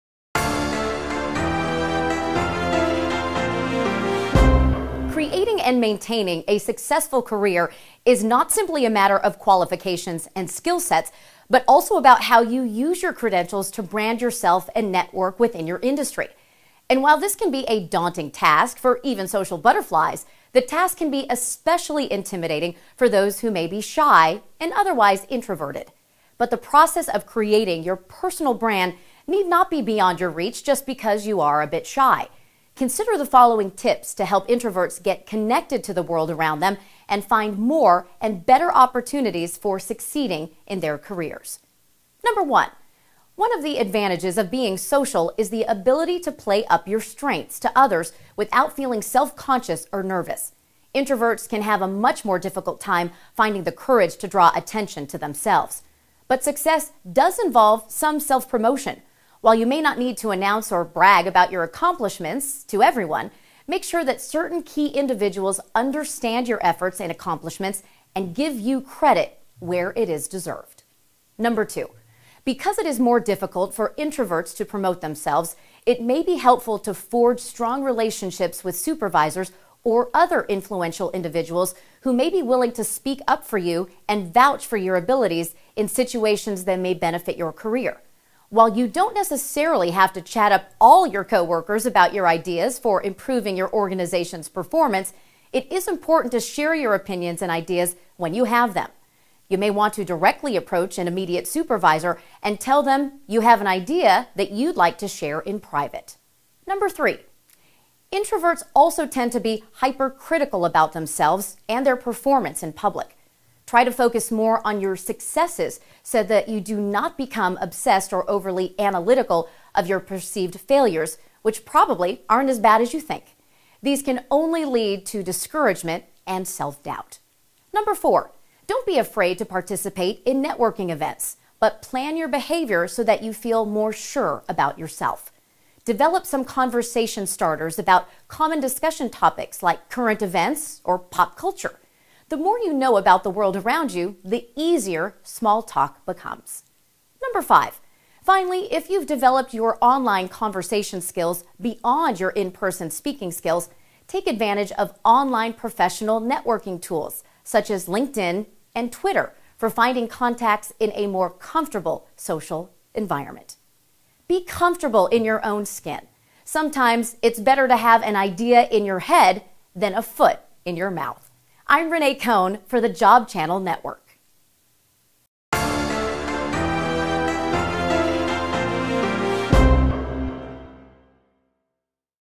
5 Tips for Introverts to Build Personal Brands
Job Channel Network's Renee Kohn has some tips to bring introverts out of their shells to sell their skills.

For more news and information about careers and the job market, visit JobChannelNetwok.com.